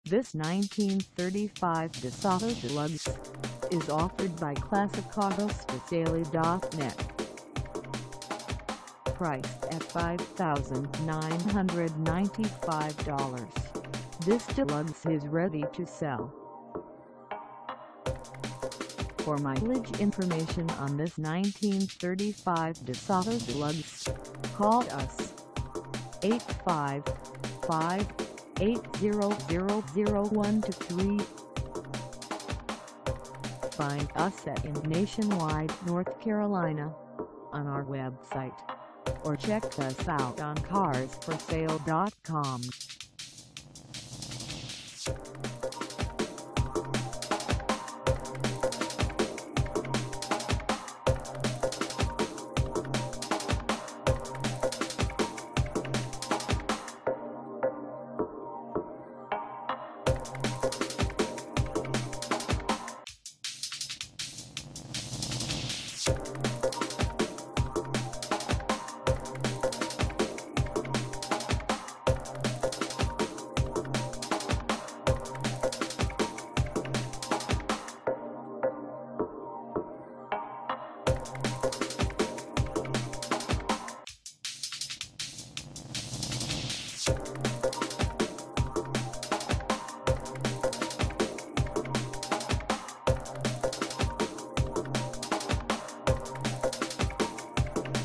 1935 DeSoto Deluxe for sale in Nationwide, NC 27603 at Clas
This 1935 DeSoto Deluxe  is for sale in Nationwide, NC 27603 at Classicautosforsale.net.

Mileage: 0
Contact Classicautosforsale.net Nationwide, NC 27603 to test drive this 1935 DeSoto Deluxe today. List your classic . The best classic car advertising on the planet!